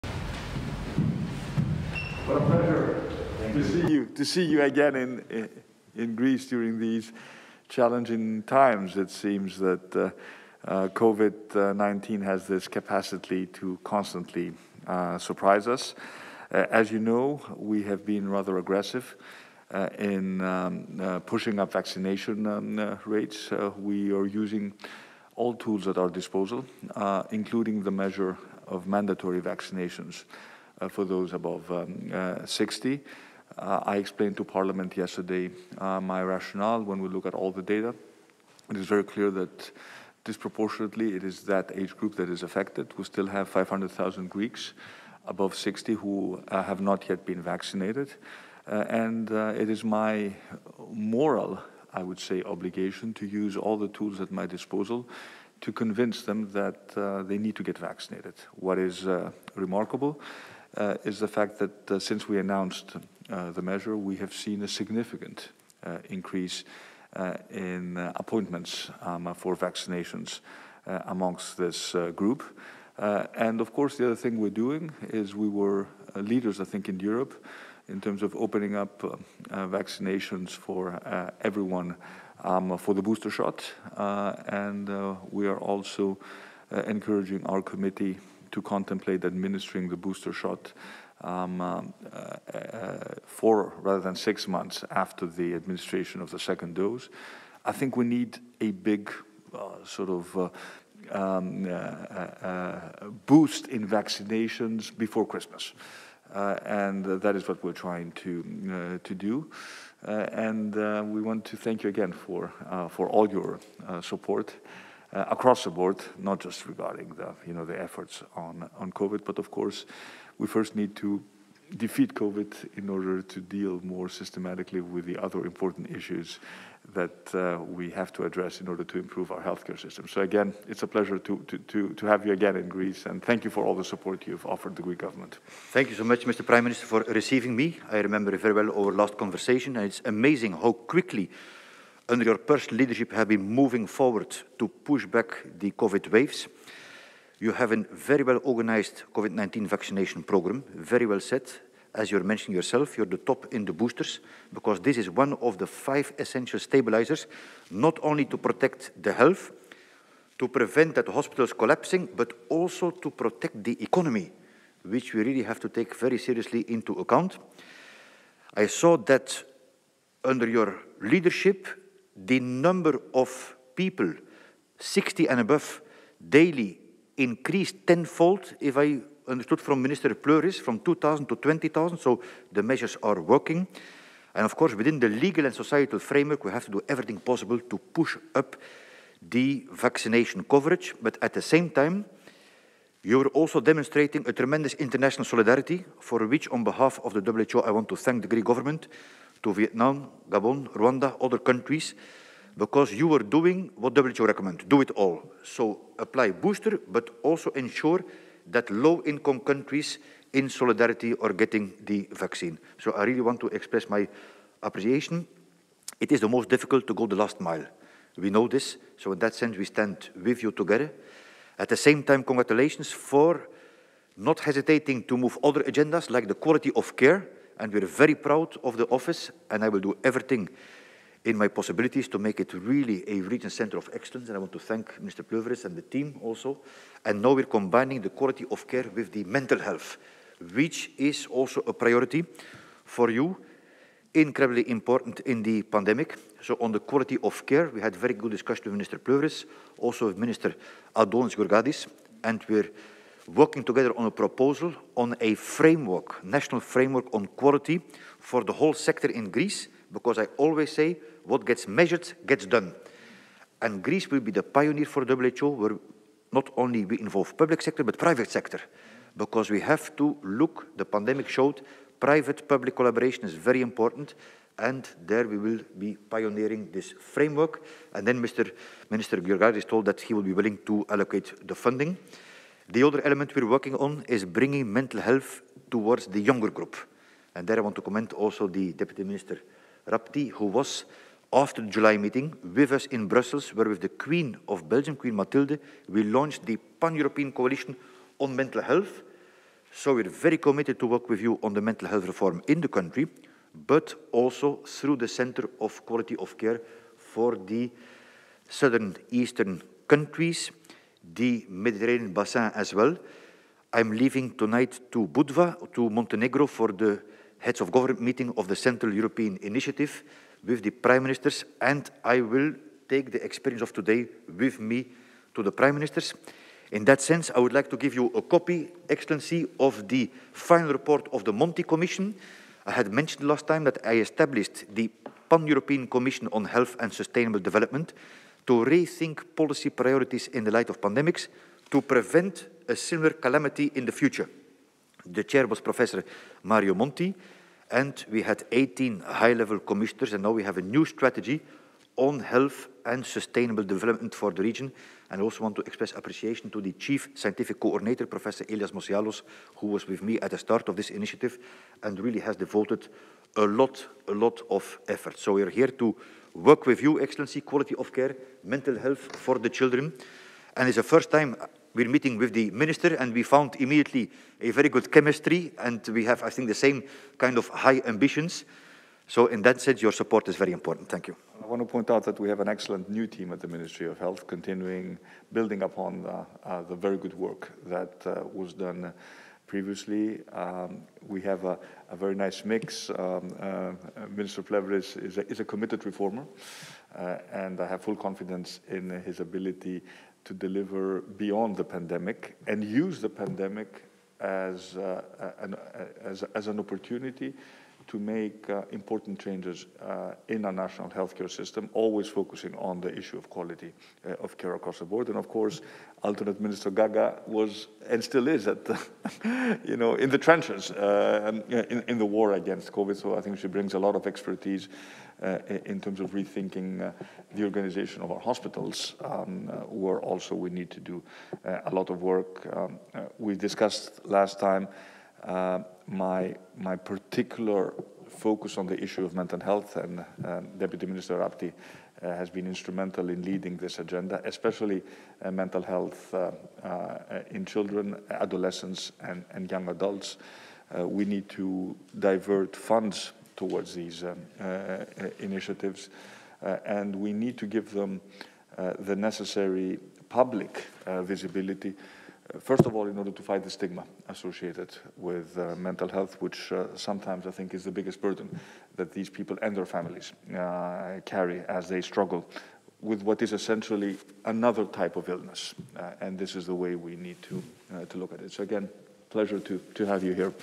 Συνάντηση Κυριάκου Μητσοτάκη με τον Περιφερειακό Διευθυντή Ευρώπης του ΠΟΥ Hans Kluge
Συνάντηση του Πρωθυπουργού Κυριάκου Μητσοτάκη με τον Περιφερειακό Διευθυντή Ευρώπης του ΠΟΥ Hans Kluge.